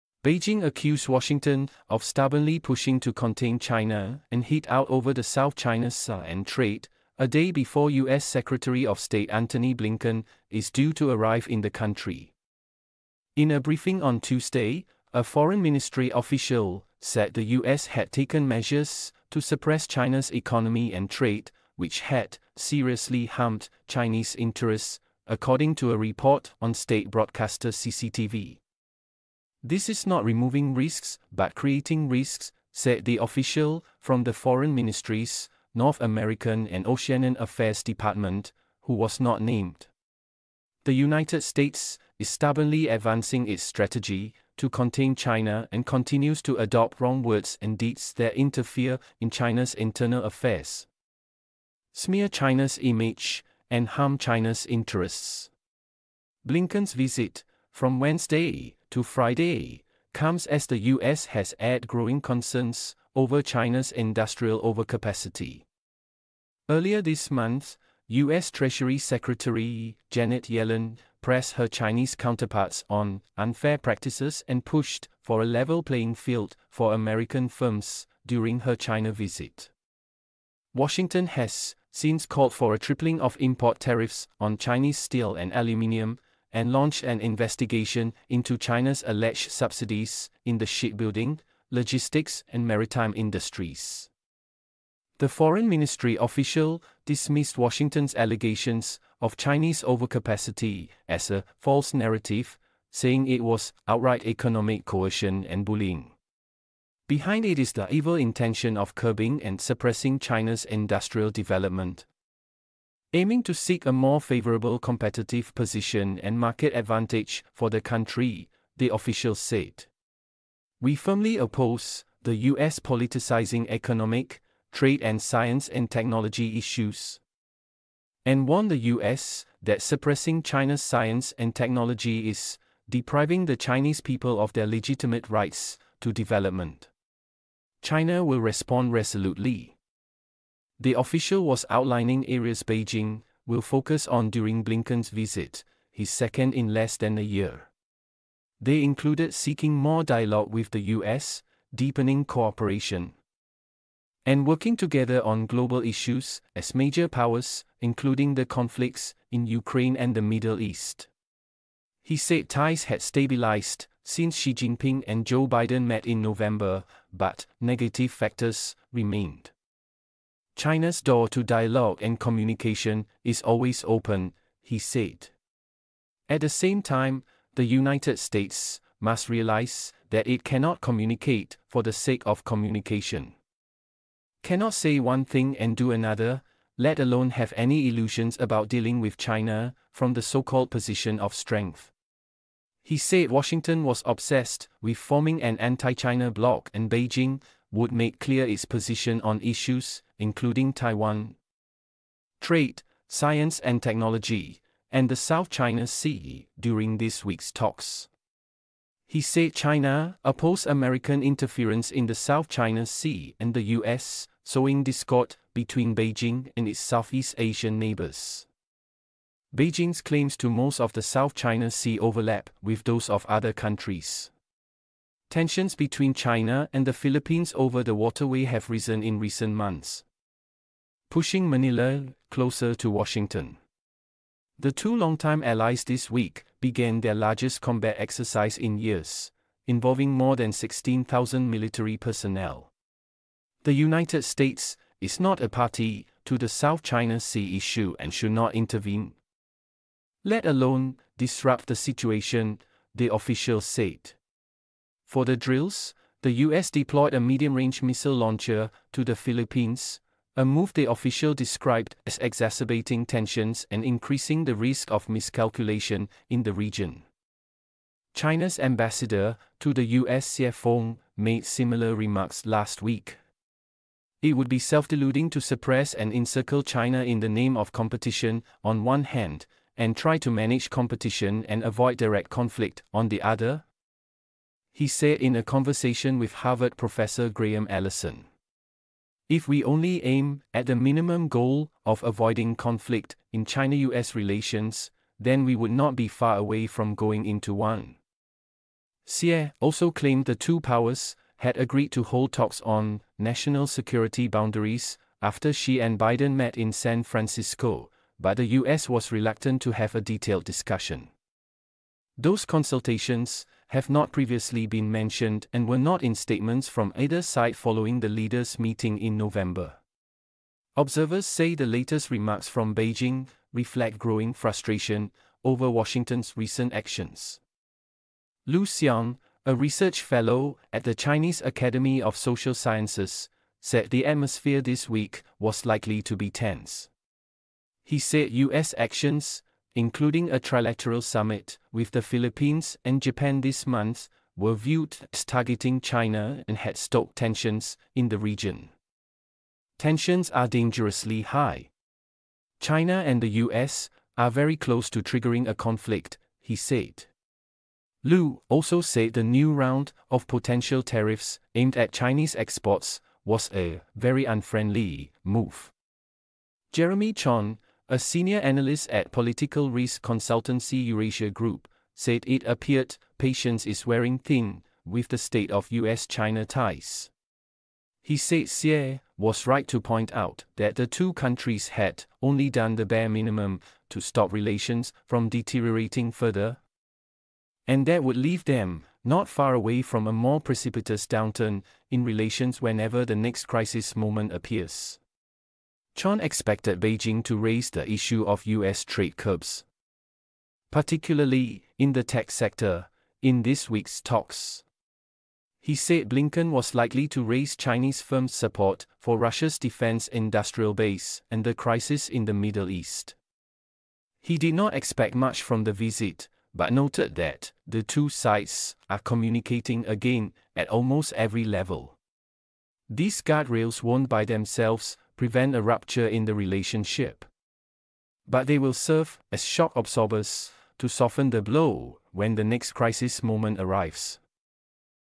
US is ‘stubbornly’ trying to contain China, Beijing says ahead of Blinken visit
💗📌 Singapore